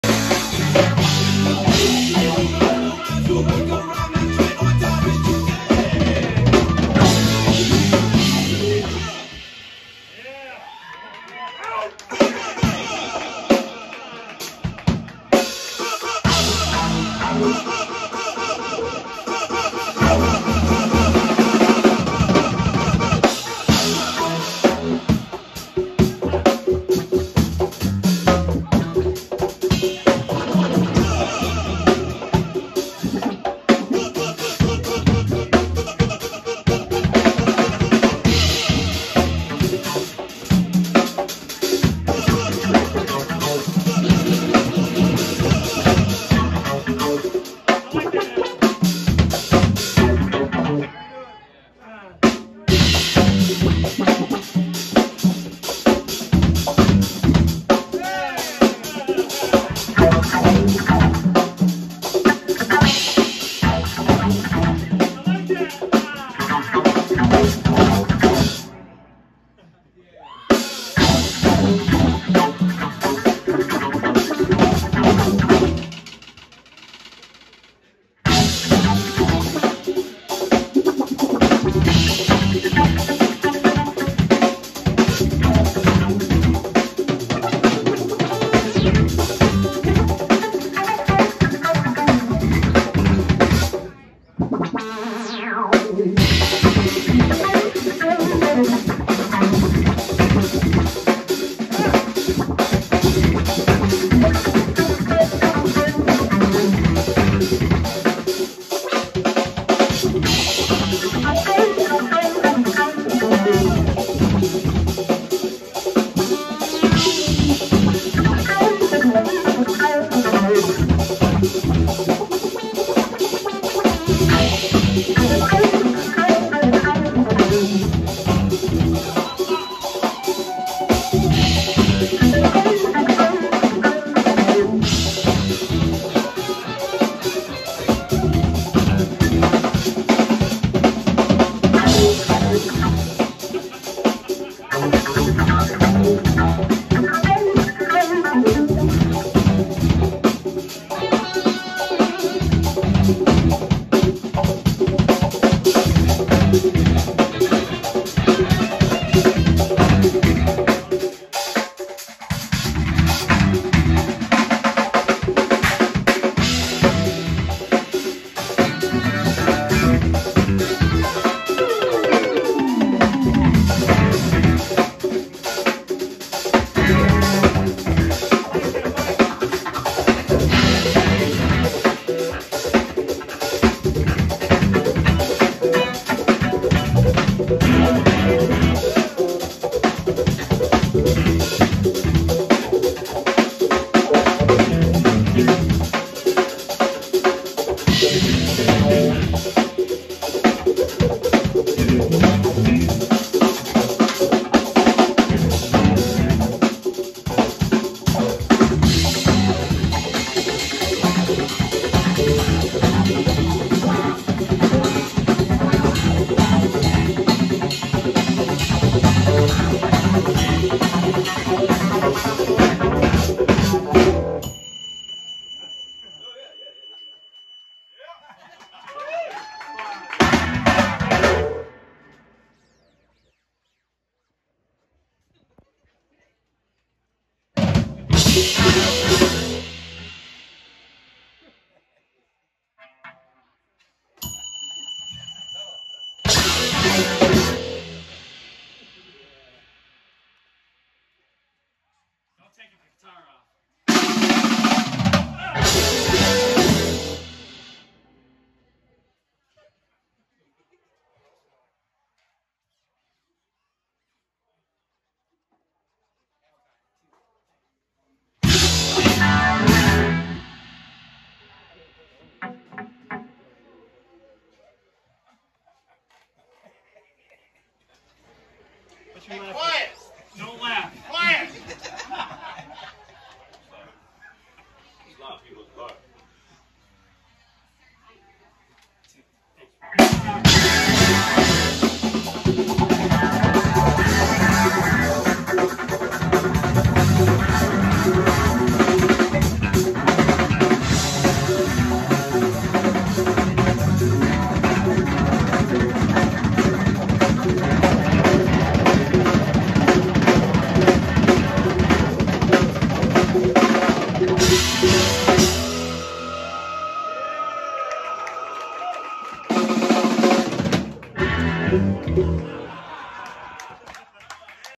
The Damone Jackson Outcome
Max davis on keys
Rodney chamberlin on bass
George Puleo on guitar
DJ Sike
Michael Ruopoli on percussion
At Central Park Plaza
October 1, 2024
Buffalo, NY